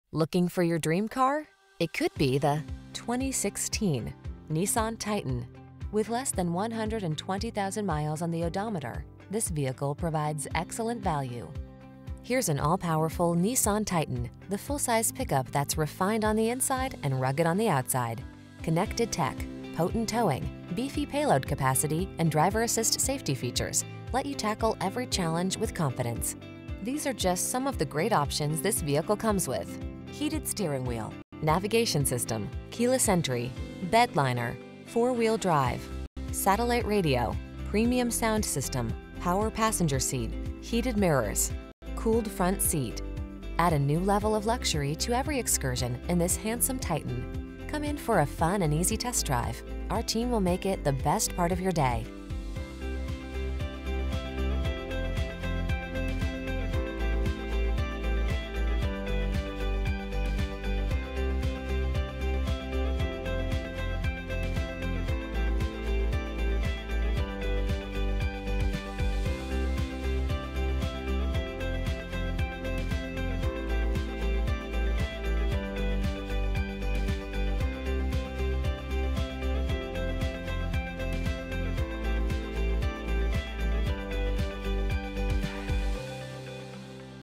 2016 Nissan Titan XD San Antonio, Austin, Houston, Dallas, Boerne, TX A23647B
2016 Nissan Titan XD Platinum Reserve

Gunn Acura
11911 IH 10 W

Call Gunn Acura at to see this vehicle and have a great experience today!Pearl White 2016 Nissan Titan XD Platinum Reserve Aisin 6-Speed Automatic 4WD 5.0L V8 Platinum Reserve